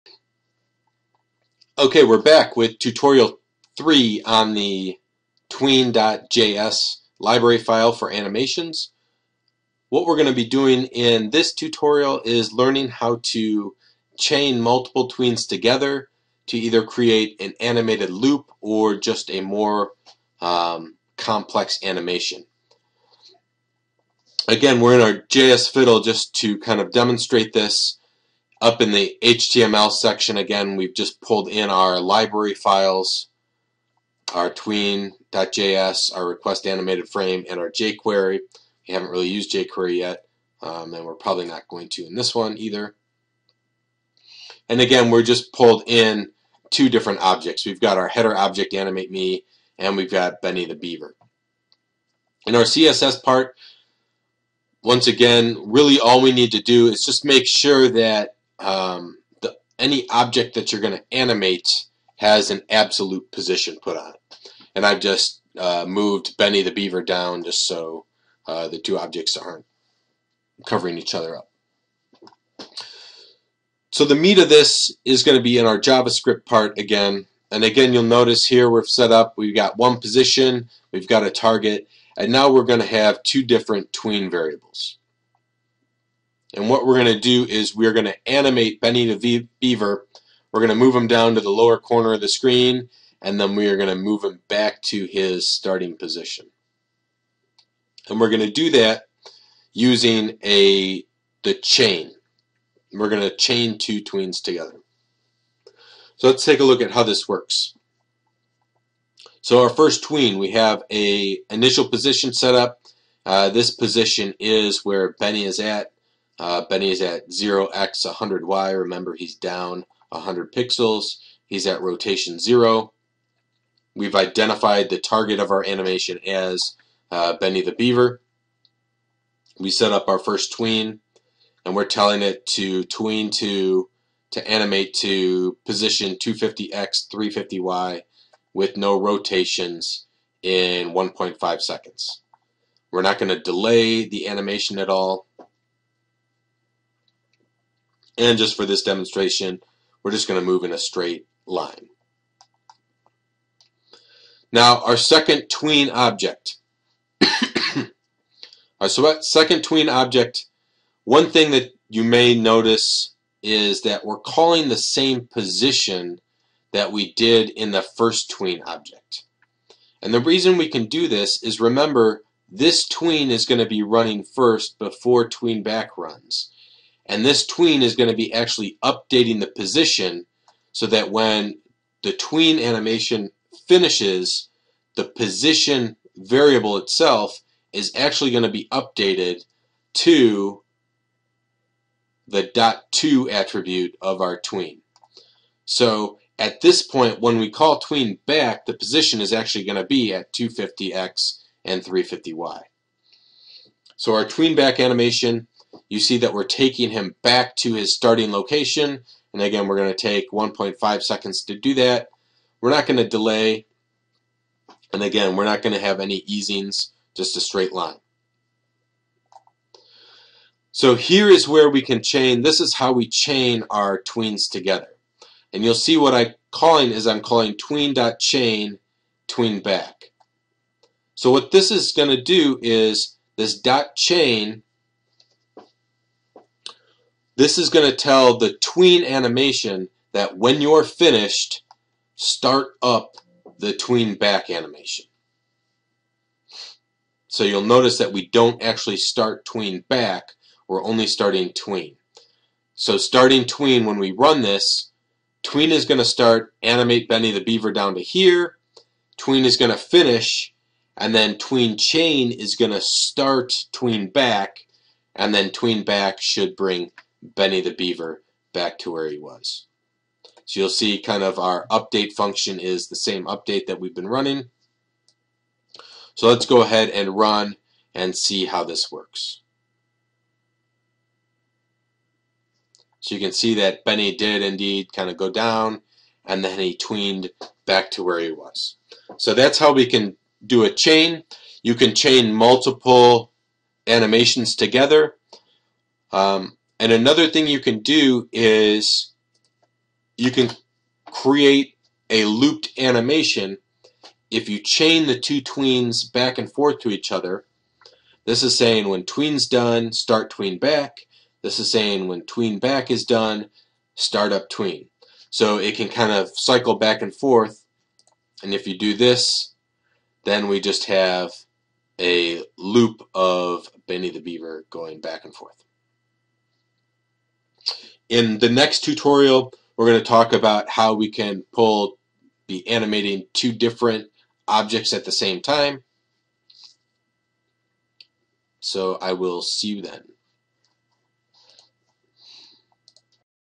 tween.js Tutorial 3: Chaining
A short tutorial on how to chain two tweens together to create more complex animations or animated loops